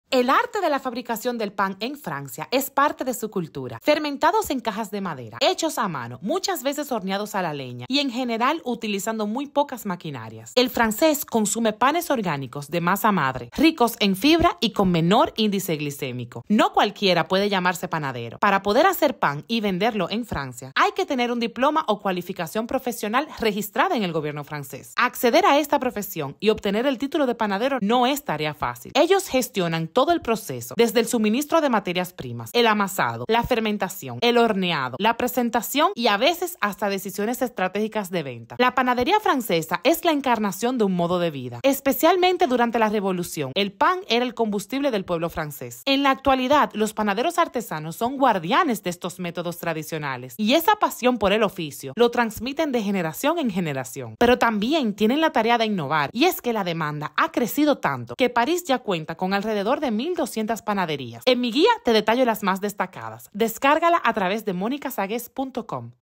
POR QUÉ EL FRANCÉS COME PAN Y NO ENGORDA
🔔¿Vienes a París?🇫🇷
Para más direcciones como esta descarga La Guía de París en monicasagues.com 📍y llega preparado(a) para conocer los mejores lugares a donde ir☑️

♥️Aquí te dejo las boulangeries que salen en el video: Atelier P1, Carton, Le BAB, Le Pain Retrouvé, Philippe Conticini, Poilâne, Polka, Ten Belles, The French Bastards, Utopie.

Si quieres probar la baguette que ganó el premio de la Mejor Baguette de París 2024, ve a la Boulangerie “Utopie”